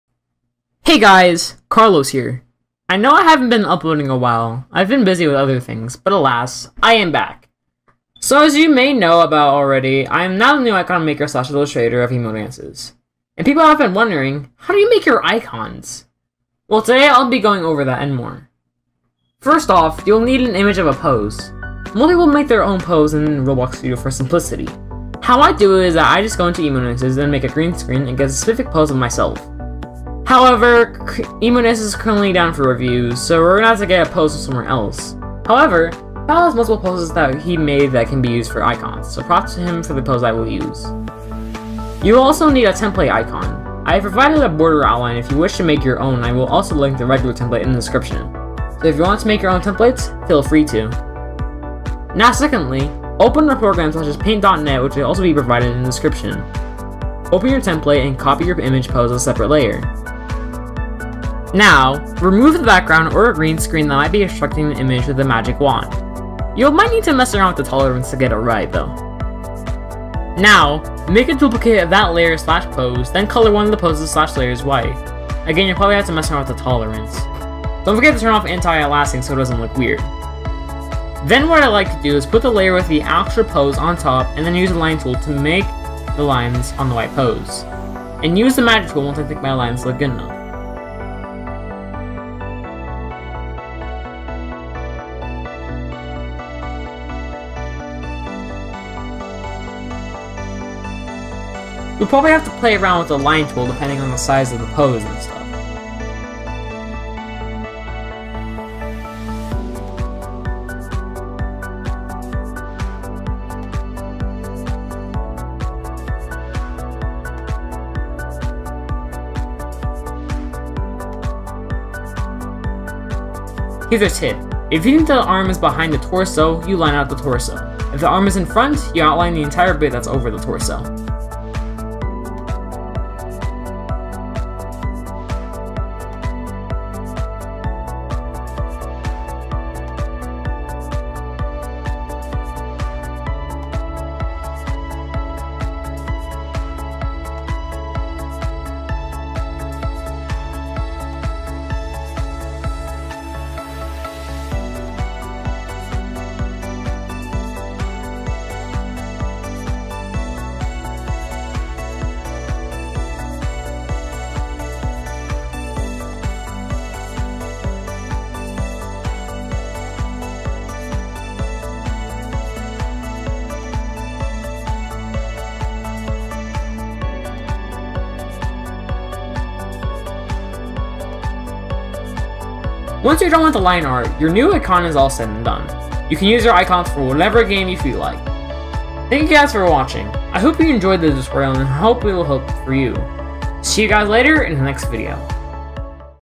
How to make your own icons! (Emote Dances)
Hello everyone! This is a tutorial on how to make your icons if needed, this will hopefully be very helpful for people who have their own emote games and need some icons.

Thanks to Isaiah on Discord for the custom template and Falco on Discord for the custom pose.